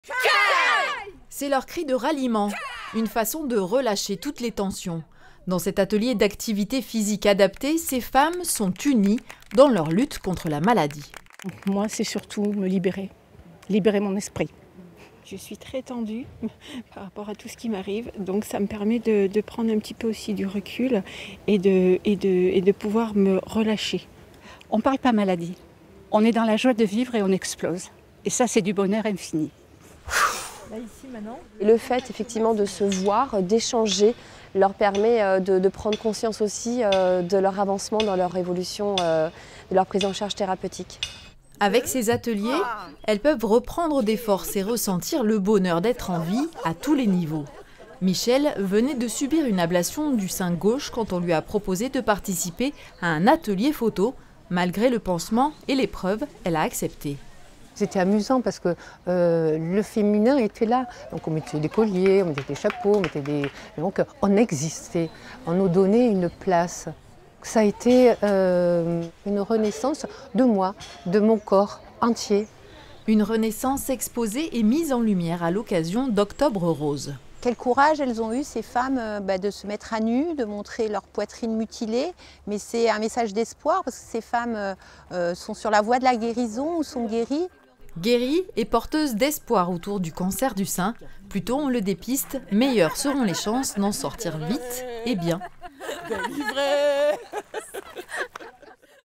Six-Fours-les-Plages se pare de rose en octobre
Octobre rose, c'est le mois pour inciter au dépistage du cancer du sein.
A Six-Fours on a décidé de mettre l'accent sur cette campagne à travers de nombreux événements. Marche, flashmob, exposition, mais aussi des ateliers thérapeutiques organisés par l'association CapSein depuis un peu plus d'un an.